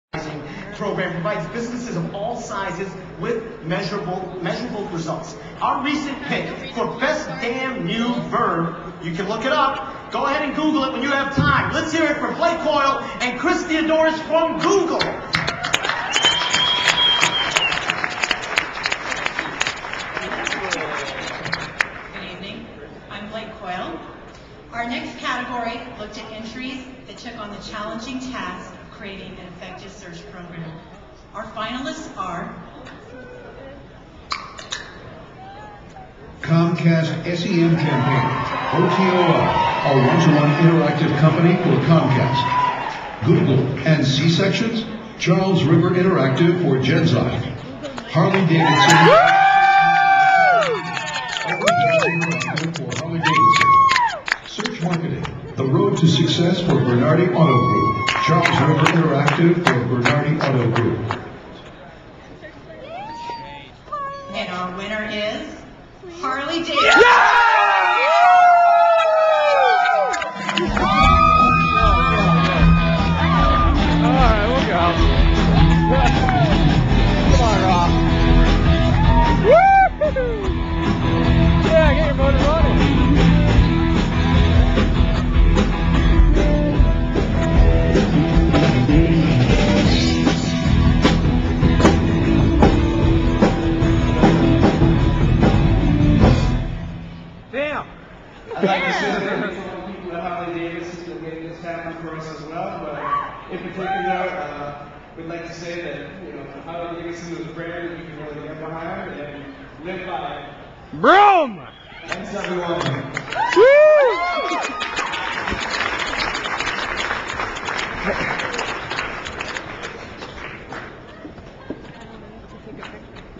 2008 Mitx Awards-Overdrive Interactive wins Best in Search for Harley-Davidson-michael orlinski
Walk with the Overdrive team as they grab there Best in Search award at the 2008 Mitx awards.